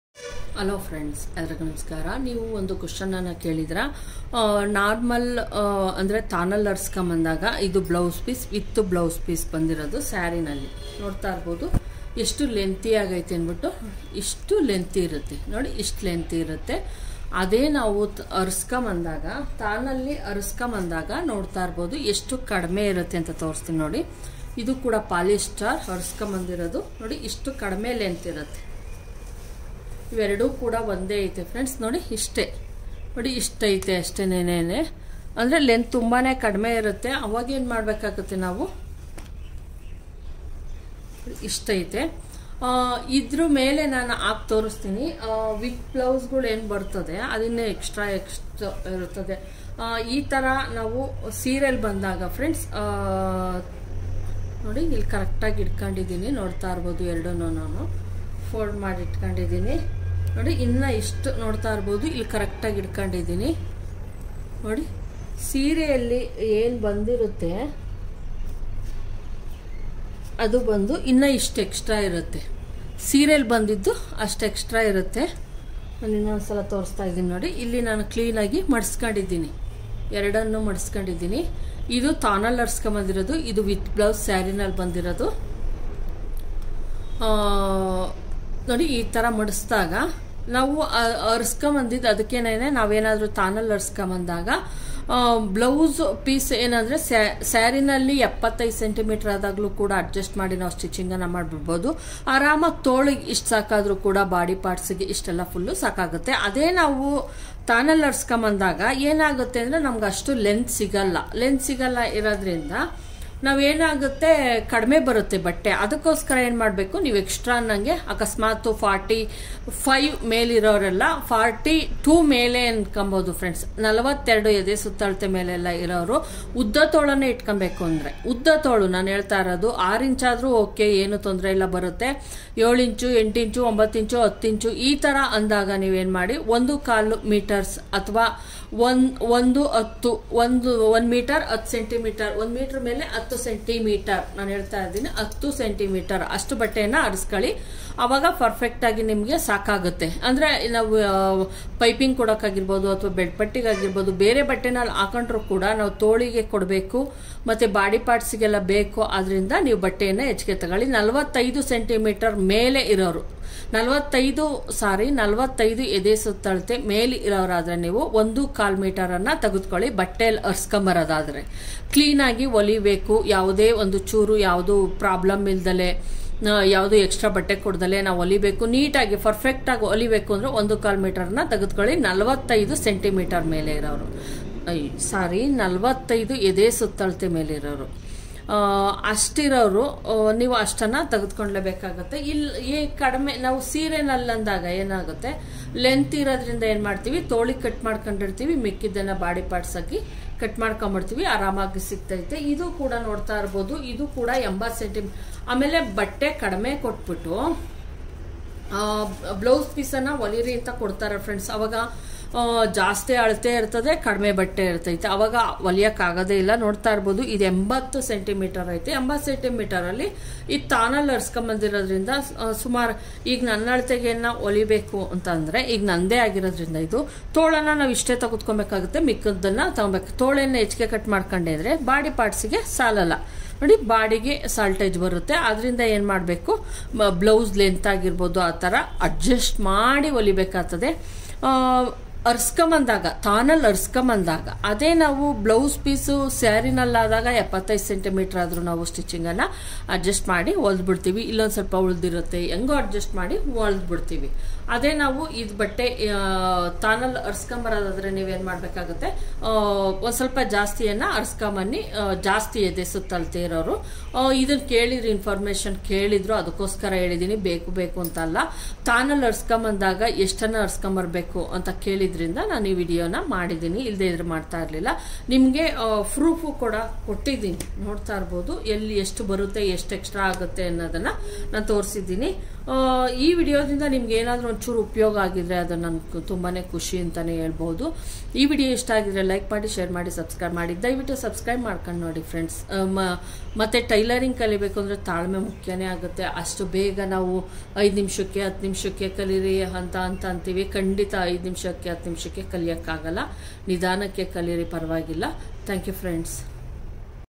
ನಾವು ಅಂಗಡಿಯಿಂದ ತರುವ ಬ್ಲೌಸ್ ಪೀಸ್ ಗು ಸೀರೆಯಲ್ಲಿ ಬಂದಿರುವ ಬ್ಲೌಸ್ ಎಷ್ಟು ವ್ಯತ್ಯಾಸ ನೋಡಿ
ನಾವು ತಾನಿನಲ್ಲಿ ಬ್ಲೌಸ್ ಪೀಸ್cut ಮಾಡಿಸಿ ಕೊಂಡು ಬರಬೇಕಾದರೆ ಎಷ್ಟು ಕೆರ್ ಫುಲ್ಲಾಗಿ ಇರಬೇಕು ಬಟ್ಟೆ ಕಡಿಮೆ ಇರುತ್ತೆ